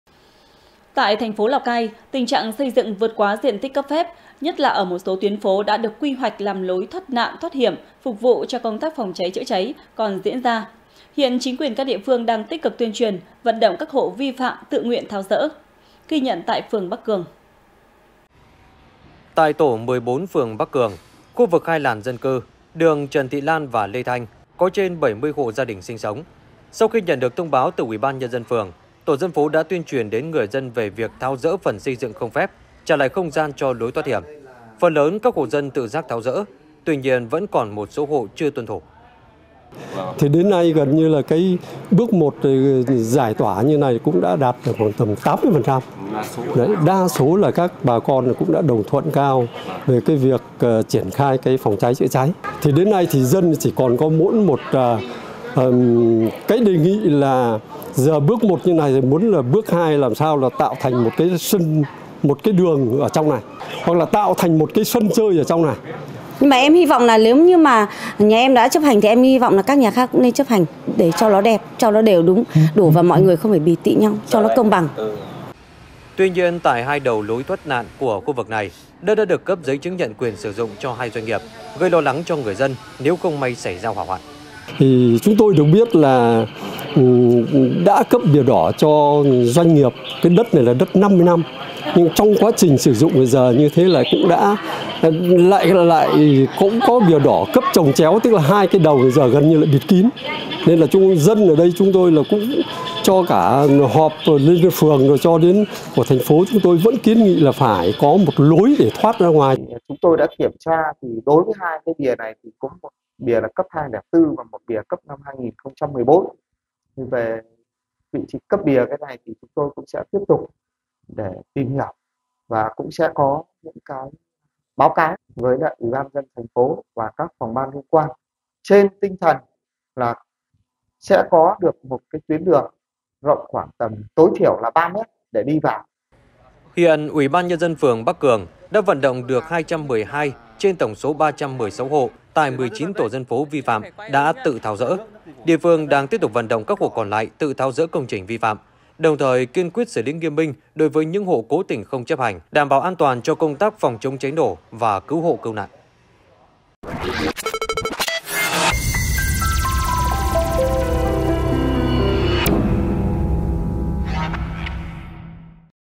Bắc Cường tháo dỡ công trình xây dựng vượt diện tích cấp phép | THLC
Laocaitv.vn - Tại thành phố Lào Cai, tình trạng xây dựng vượt quá diện tích cấp phép, nhất là ở một số tuyến phố đã được quy hoạch làm lối thoát nạn, thoát hiểm phục vụ cho công tác phòng cháy, chữa cháy còn diễn ra. Hiện, chính quyền các địa phương đang tích cực tuyên truyền, vận động các hộ vi phạm tự nguyện tháo dỡ. Ghi nhận tại phường Bắc Cường.
Đài Phát thanh - Truyền hình Lào Cai